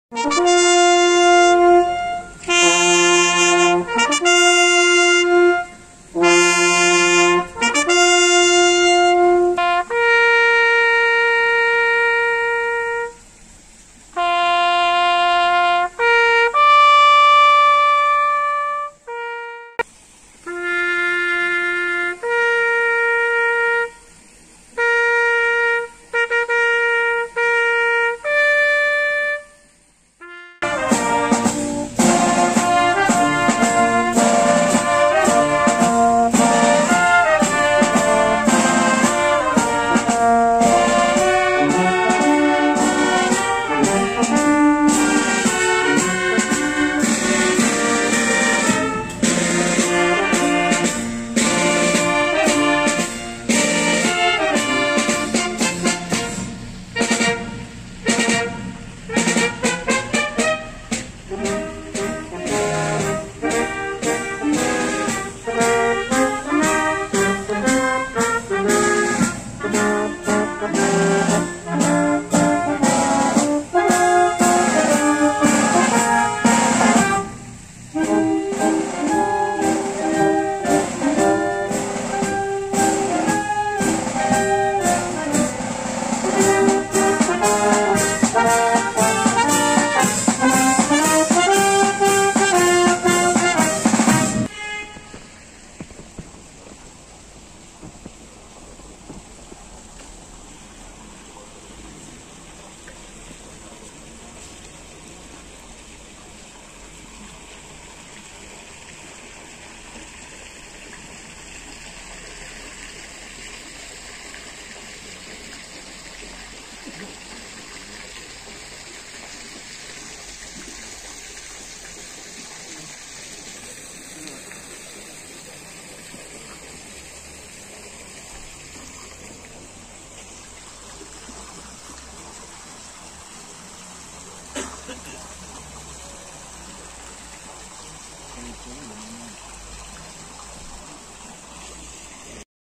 Celebrazione per il 77° anniversario della Liberazione
Ceglie Messapica | 25•04•2022 |
Video e montaggio di Pierangelo Lerna